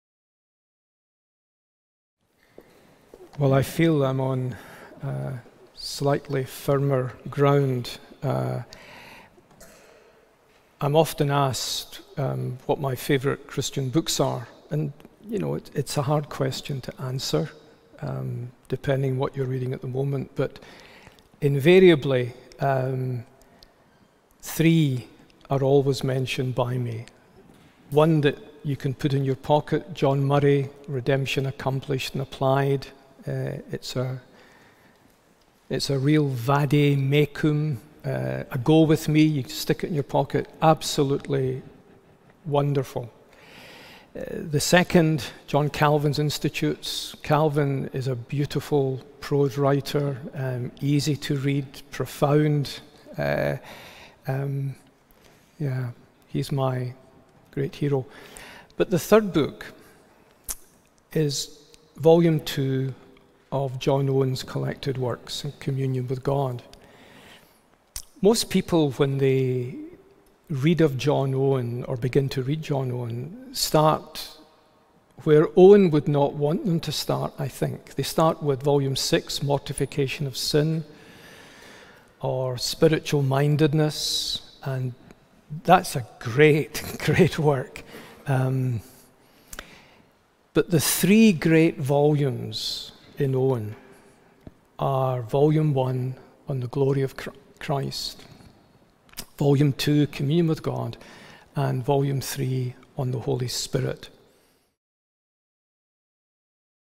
3 Must-Read Theological Books | Ian Hamilton | Westminster Q&A
Join pastor and author Ian Hamilton for the next in our bitesize series: Westminster Q&A.